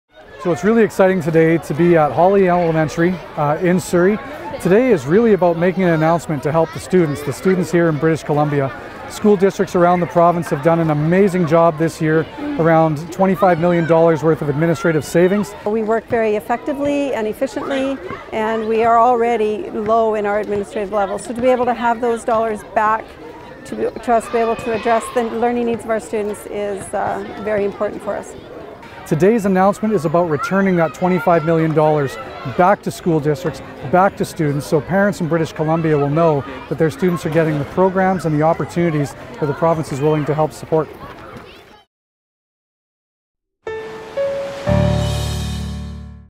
Administrative savings redirected for students
The Government of British Columbia is redirecting $25 million in administrative savings from school districts back into frontline services for students.